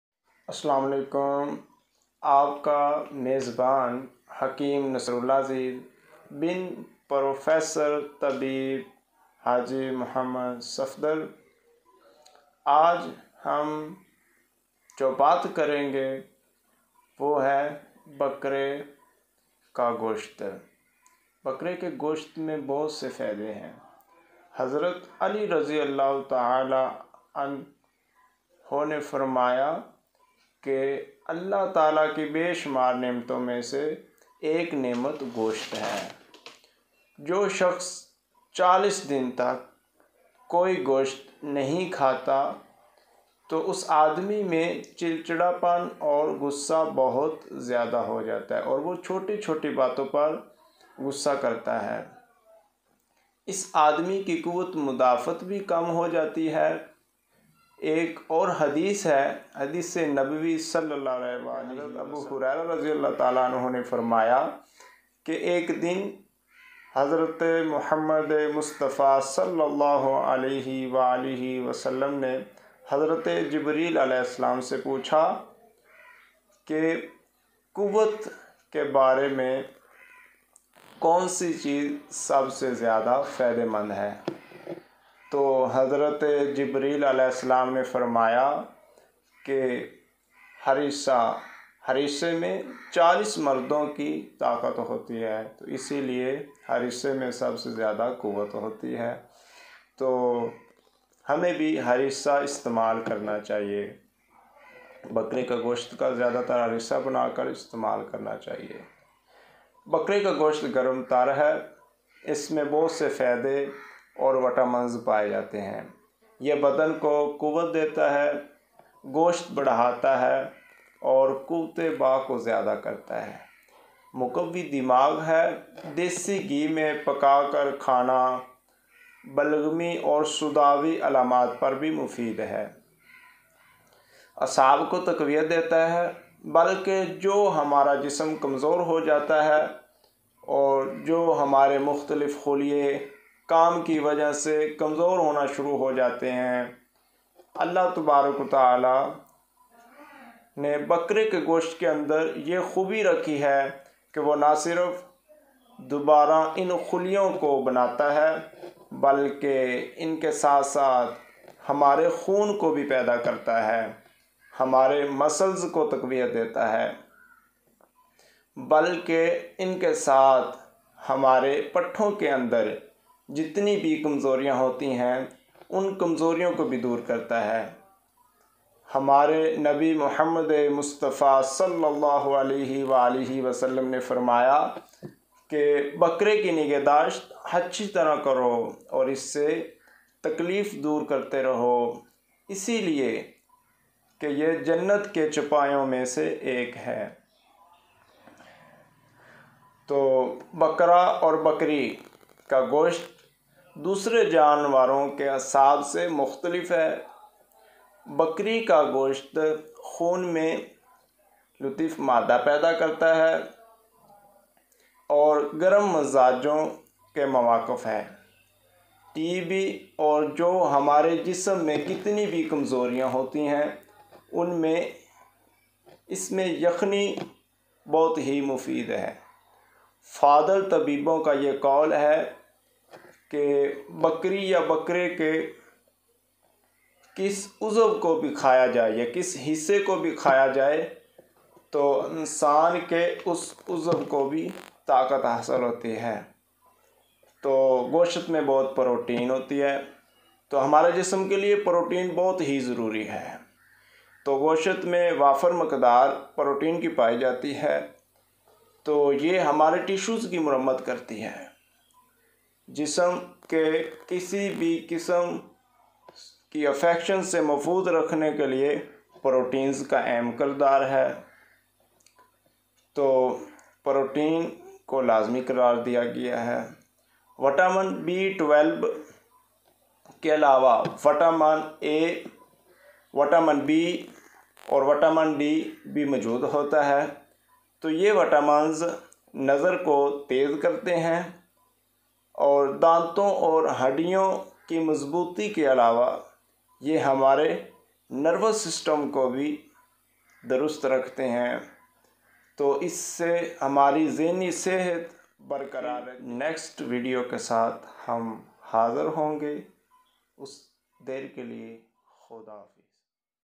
Sexual weakness and treatment through food Mardana kamzori ka elaj gosht k faiday
Sexual weakness is common in people today.it can be cured through food too.mutton and hareesa are treatment of this weakness prescribed in Islamic Tibb..